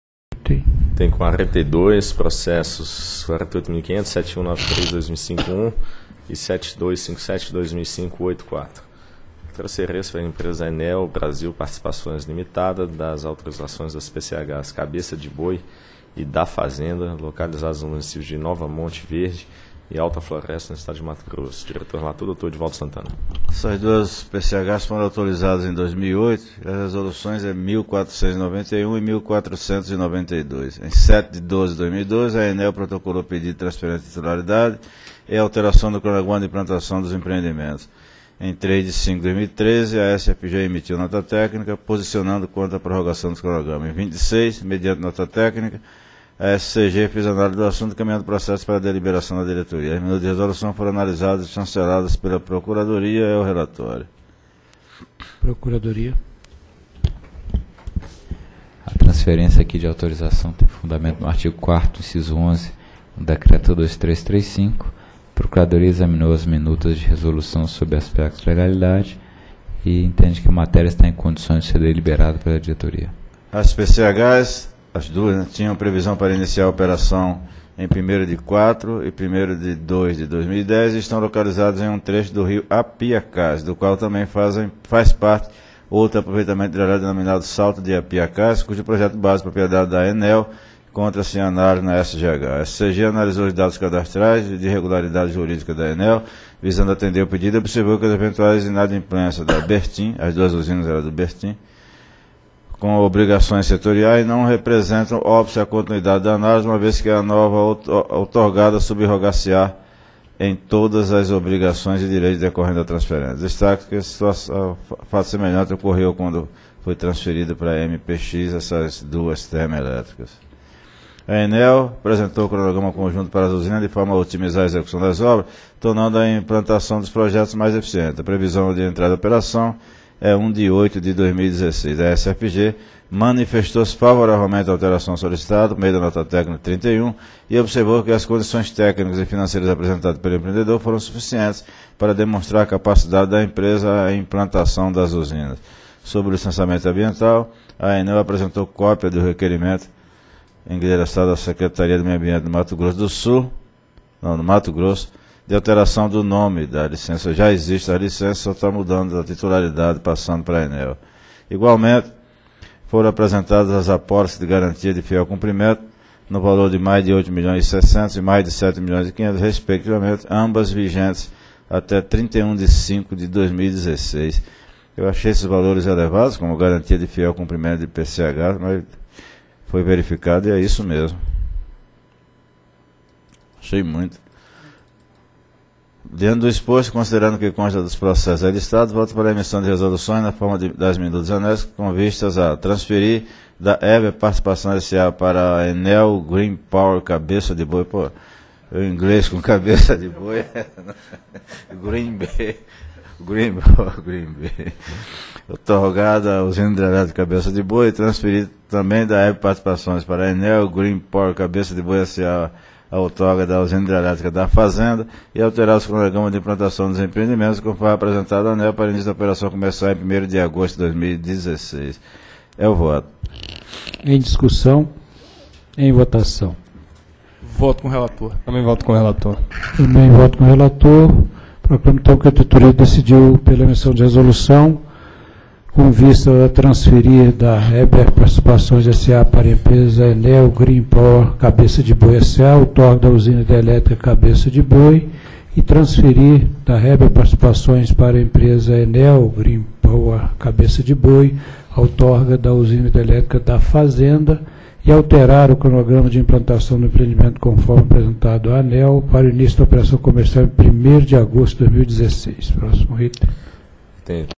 Item 42 - 25ª Reunião Pública Ordinária ANEEL 09/07/2013
Item 42. Processo: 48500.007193/2005-01 e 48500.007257/2005-84.
Assunto: Transferência, para a empresa Enel Brasil Participações Ltda., das autorizações das Pequenas Centrais Hidrelétricas -- PCHs Cabeça de Boi e Da Fazenda, localizadas nos municípios de Nova Monte Verde e Alta Floresta, no estado do Mato Grosso. Área Responsável: Superintendência de Concessões e Autorizações de Geração -- SCG.